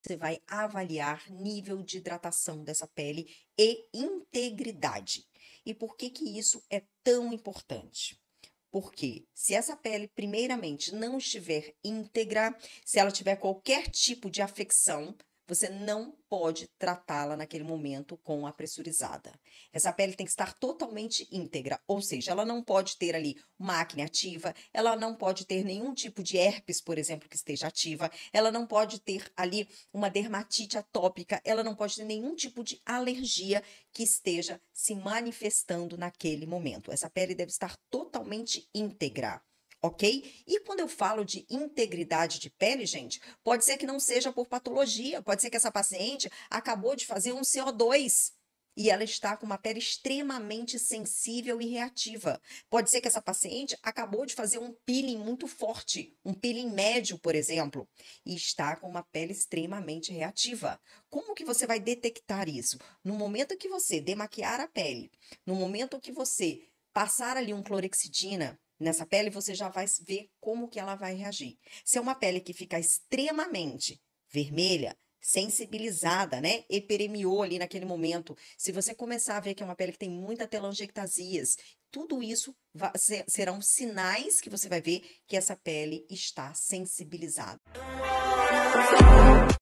NÃO FAÇA ISSO nesse tipo de pele ❌
Se você deseja TER SUCESSO em seus tratamentos estéticos e se tornar uma referência na sua região, é INDISPENSÁVEL que você faça essa avaliação na sua paciente  👀

Se essa pele está vermelha e sensibilizada, esse é o primeiro sinal de que você não deve realizar nenhum procedimento naquele momento, com a Pressurizada ❌